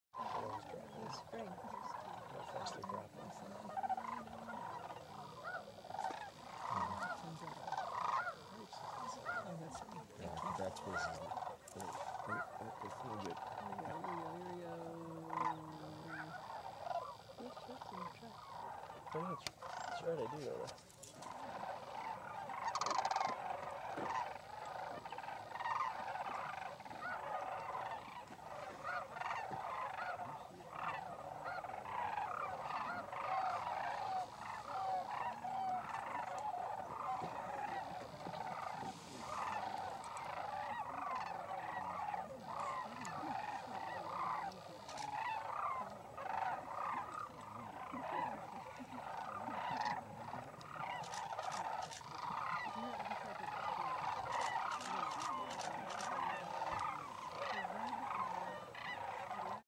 Cranes!
This is a LOT of sandhill cranes.  They com through here every year in late January.